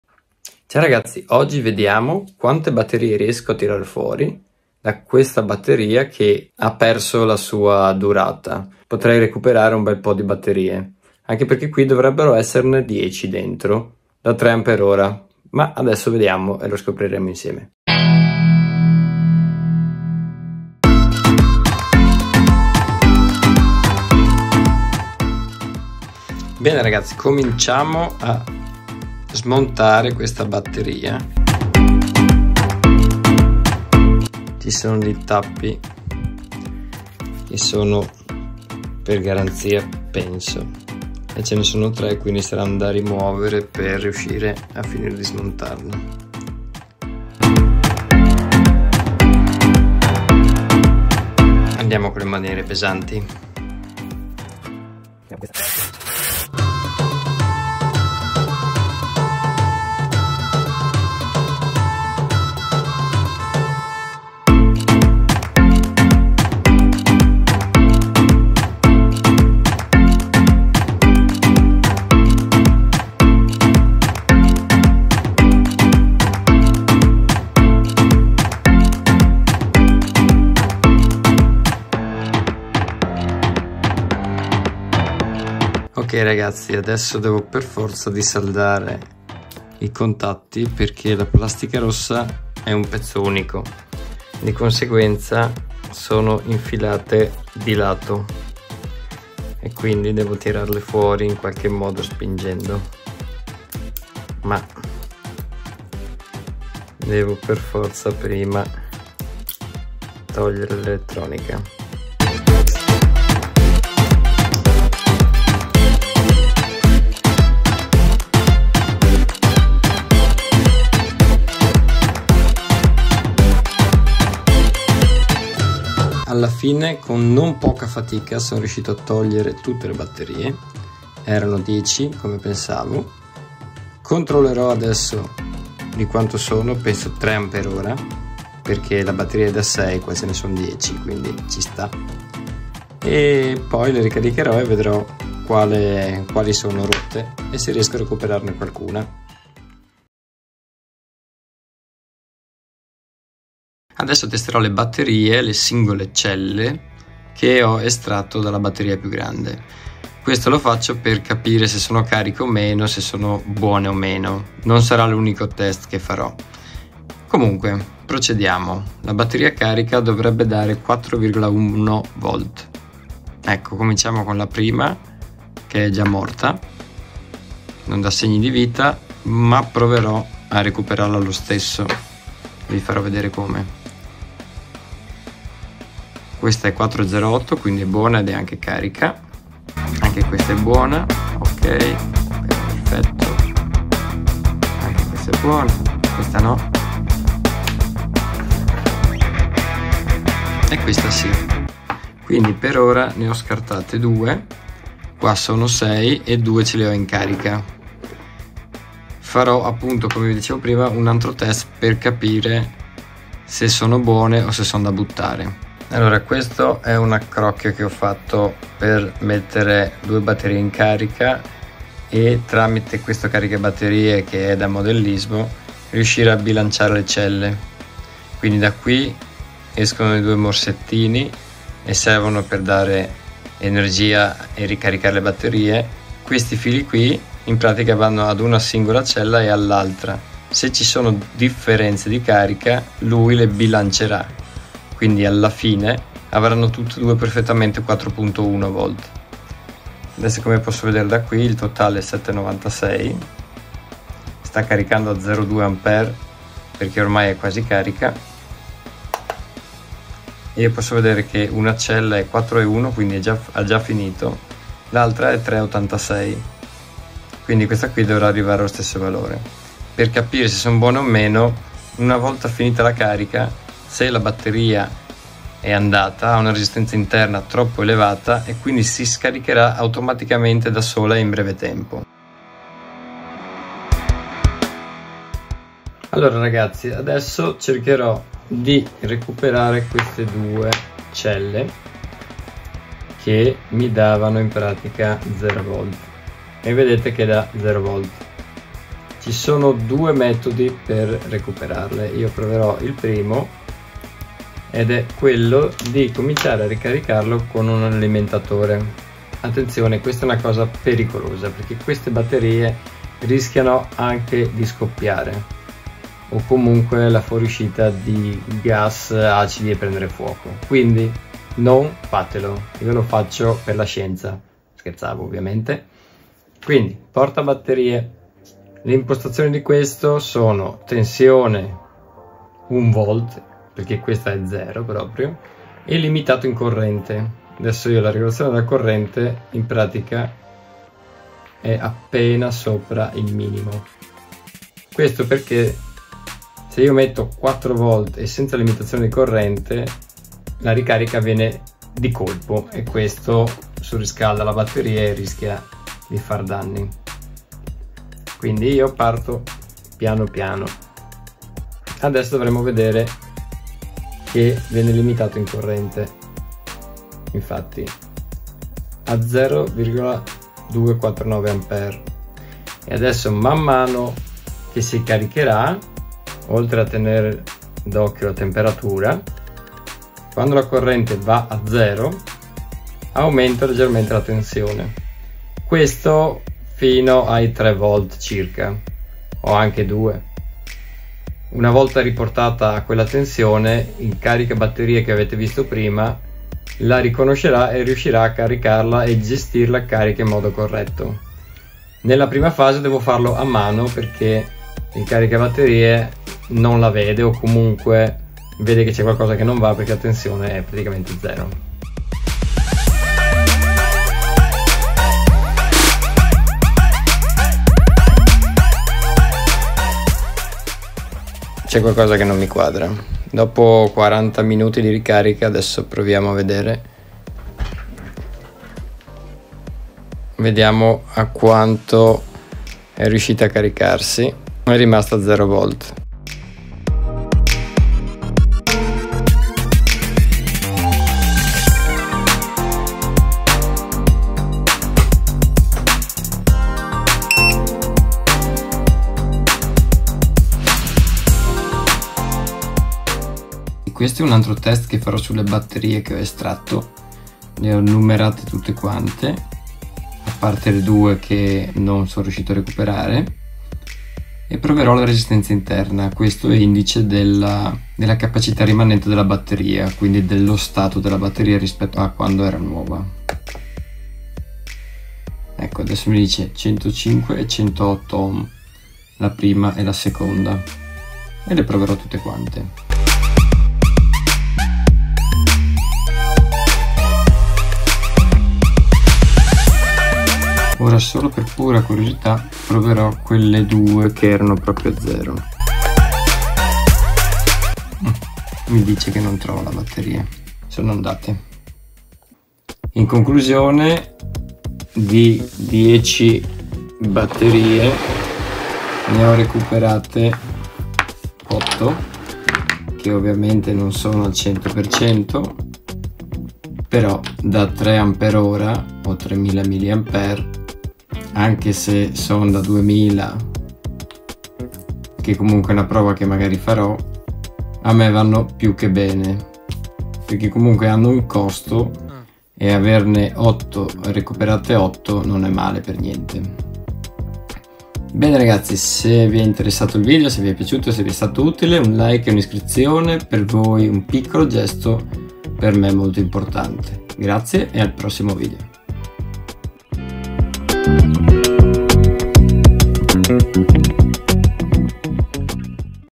Batteria utensile finita? Batterie Li-Ion gratis! 😎
Ciao ragazzi!

Attenzione!
--- Queste batterie possono esplodere!! ---

00:00 Introduzione
00:20 Apro la batteria
02:30 Test e ricarica batterie
04.42 Tentativo recupero batterie danneggiate
07:30 Test resistenza interna e conclusioni

In questo video vi faccio vedere dove trovare gratis le batterie Li-Ion 18650, come avevo già fatto in quest'altro video:

Come faccio il test per capire se sono in buono stato e come faccio per recuperarle se sembrano ormai "finite".

Un secondo metodo per recuperarle è quello di ruotare la "griglia" del positivo. Ho deciso però di non farlo per questioni di sicurezza della batteria stessa. Devo informarmi di più per capire quali metodi usare e i rischi collegati.

Ovviamente le batterie recuperate non saranno al 100%, ma vanno più che bene per l'utilizzo normale che faccio io come ad esempio per alimentare il tester che vedete nel video, per il radiocomando di droni, visori e per ripristinare la batteria di un vecchio portatile.

Gli utilizzi sono molteplici, ormai questo tipo di batterie sono presenti in moltissimi prodotti.
Queste, anche se usurate, sono originali e di ottima fattura (Samsung).
Secondo me migliori di quelle a basso prezzo cinesi che tra l'altro possono essere pericolose e con dati di targa, come la capacità, non reali.

Qui sotto i 2 video che ho fatto dove appunto uso le batterie per recuperare e potenziare una racchetta per le zanzare e recuperare un avvitatore:

Grazie!

Se avete qualcosa da chiedermi sarò felice di rispondervi se posso.
Se avete qualche suggerimento fatevi avanti!

Grazie ancora, ciao!